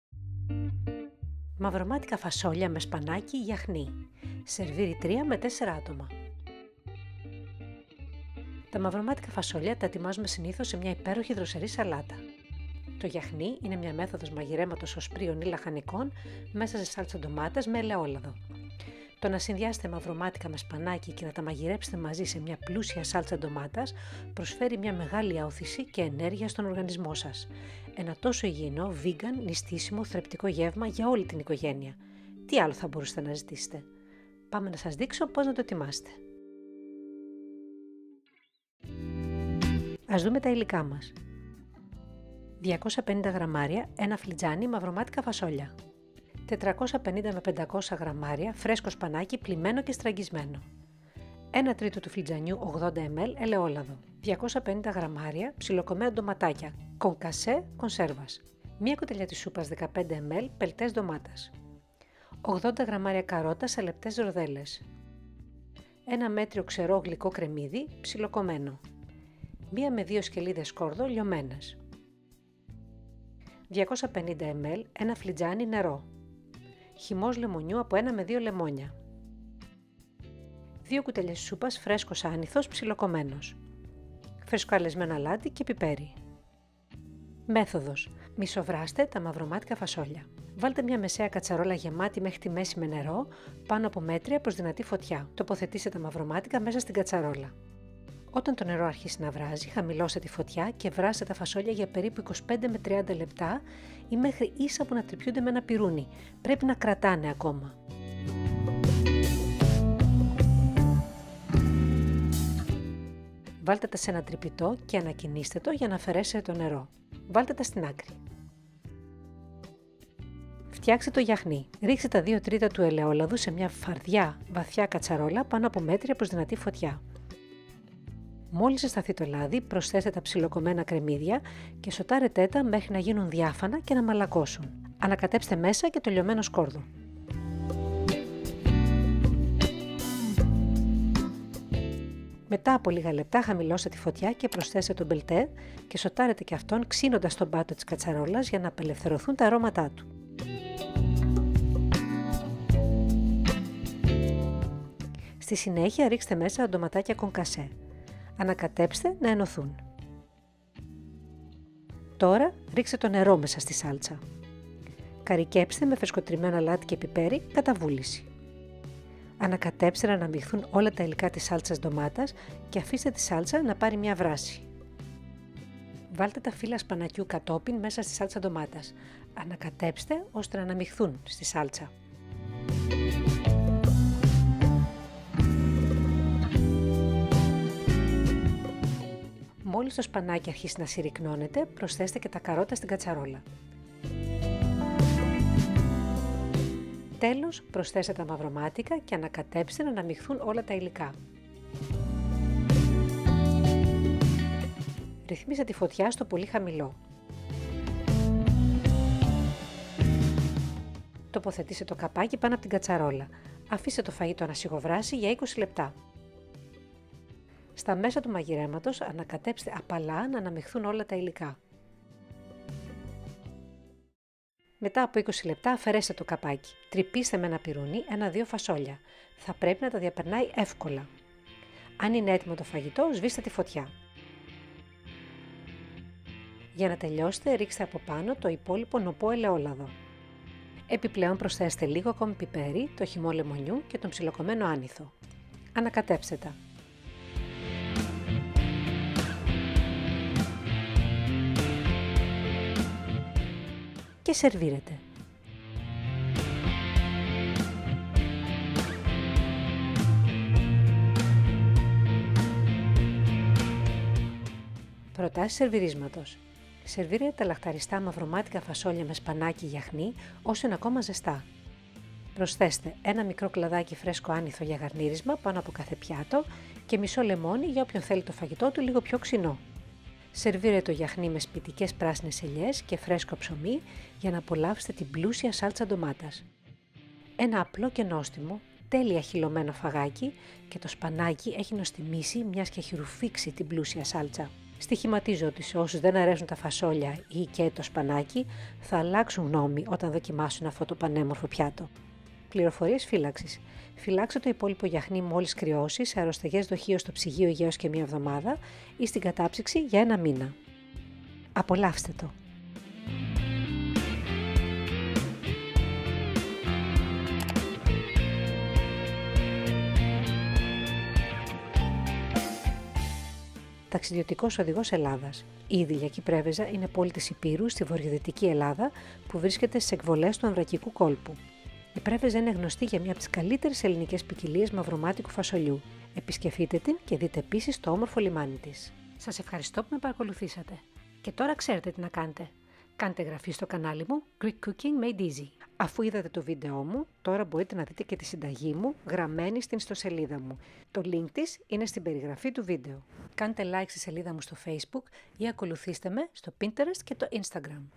Black eyed Peas w/ Spinach Yiahni (Mavromatika Tomato sauce) - Μαυρομάτικα Φασόλια με Σπανάκι Γιαχνί
Black eyed Peas with Spinach Yiahni (in Tomato sauce) – Mavromatika Fasolia me Spanaki Yiahni
BY: Greek Cooking Made Easy
You can also check this recipe and all of my recipes in text (also in Greek) on my Website below!
Μπορείτε επίσης να βρείτε αυτή τη συνταγή μου όπως και όλες τις άλλες, γραμμένες και στα Ελληνικά, στον Ιστότοπό μου, στο παρακάτω λίνκ!
NOTE: This video is bilingual. The recipe is written in English. Turn volume ON to hear the recipe read out in Greek 🔈.
ΣΗΜΕΙΩΣΗ: Αυτό το βίντεο είναι δίγλωσσο. Η συνταγή είναι γραμμένη στα Αγγλικά. Ανεβάστε την ένταση του ήχου στο βίντεο, για ακουστική περιγραφή της συνταγής στα Ελληνικά 🔈.

Serves 3-4 persons
Greeks call them “Mavromatika fasolia” i.e. black eyed BEANS-because this is what they actually are and the legume family they belong to.
Black Eyed Peas are usually prepared as a wonderful, refreshing Salad. Yiahni is a Greek method of preparing legumes or vegetables cooked in a rich tomato sauce with Olive oil.
Combining black eyed peas with spinach and cooking them together in a rich tomato sauce is like a boost to your system. Such a healthy, vegan, Lenten, nutritious meal for the whole family!! What else could you ask for!
Let me show you how to prepare it.
Suitable for vegans and fasting.

INGREDIENTS:
• 250 gr / 9 oz / 1 cup Black Eyed Peas
• 450-500 gr / 1 lb-1 lb 2 oz Fresh Spinach, washed and drained
• 1/3 cup / 80 ml Olive Oil
• 250 gr / 9 oz chopped Tomatoes, canned, concassé
• 1 tbsp. / 15 ml Tomato Paste (Puree)
• 80 gr / 2.8 oz Carrots, in thin rounds
• 1 medium, Sweet Onion, finely chopped
• 1-2 Garlic Cloves, minced
• 250 ml / 1 cup Water
• Lemon Juice from 1-2 lemons
• 2 tbsp. fresh Dill, chopped
• Ground Salt and Pepper

Μαυρομάτικα Φασόλια με Σπανάκι Γιαχνί
Σερβίρει 3-4 άτομα
Τα Μαυρομάτικα Φασόλια τα ετοιμάζουμε συνήθως σε μια υπέροχη, δροσερή σαλάτα. Το Γιαχνί είναι μια μέθοδος μαγειρέματος οσπρίων ή λαχανικών μέσα σε σάλτσα ντομάτας με ελαιόλαδο.
Το να συνδυάσετε μαυρομάτικα με σπανάκι και να τα μαγειρέψετε μαζί σε μια πλούσια σάλτσα ντομάτας προσφέρει μια μεγάλη ώθηση και ενέργεια στον οργανισμό σας. Ένα τόσο υγιεινό, βίγκαν, νηστίσιμο, θρεπτικό γεύμα για όλη την οικογένεια!! Τι άλλο θα μπορούσατε να ζητήσετε!
Πάμε να σας δείξω πώς να το ετοιμάσετε.
Κατάλληλο για βίγκαν και νηστεία.

ΥΛΙΚΑ:
• 250 γρ / 9 oz / 1 φλ. Μαυρομάτικα Φασόλια
• 450-500 γρ / 1 lb-1 lb 2 oz φρέσκο ​​Σπανάκι, πλυμένο και στραγγισμένο
• 1/3 φλ. / 80 ml Ελαιόλαδο
• 250 γρ / 9 oz ψιλοκομμένα Ντοματάκια (κονκασέ κονσέρβας)
• 1 κ.σ. / 15 ml πελτές Ντομάτας
• 80 γρ / 2,8 oz Καρότα, σε λεπτές ροδέλες
• 1 μέτριο, ξερό, Γλυκό Κρεμμύδι, ψιλοκομμένο
• 1-2 Σκελίδες Σκόρδο, λιωμένες
• 250 ml / 1 φλιτζάνι Νερό
• Χυμός λεμονιού από 1-2 λεμόνια
• 2 κ.σ. φρέσκος Άνηθος, ψιλοκομμένος
• Φρεσκοαλεσμένο αλάτι και πιπέρι